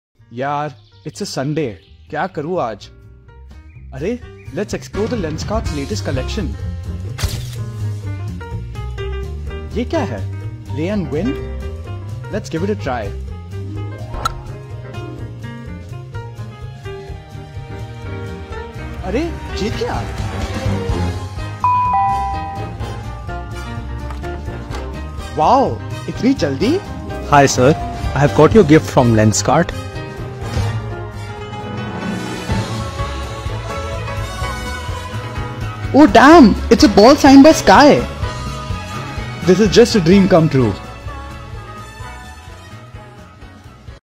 Get A Change To Win SuryaKumarYadav Signed Merchandise
Get your game face on because we’ve got more than just bragging rights in store 🏏

It's your chance to win a signed merchandise by SKY himself 🖋️ All you have to do is download the Lenskart app and play the game.
The next winner could be you, so what are you waiting for? PLAY NOW! 🎉🙌

Winners are to be announced daily on the LenskartApp.